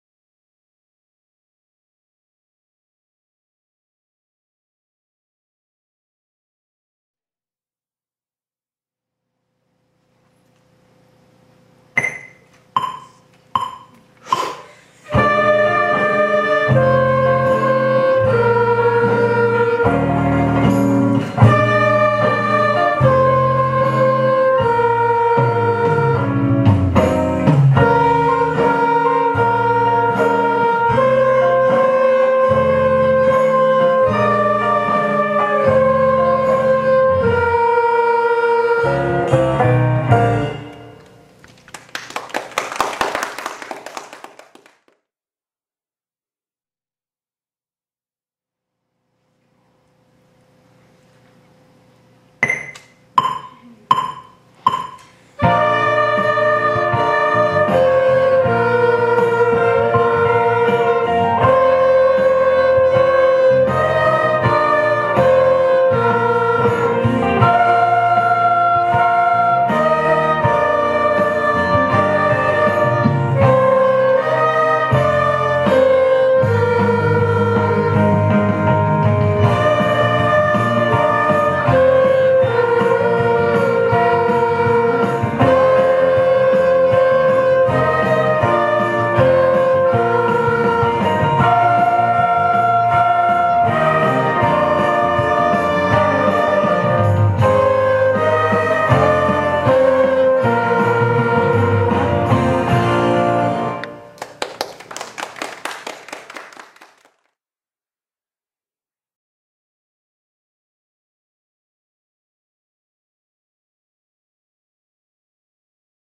Vale MS - Beg. Flute, Ob, Bassoon - Fall 2019
Vale Middle School  - Beg. Flute, Ob, Bassoon - Fall 2019 in a Virtual Concert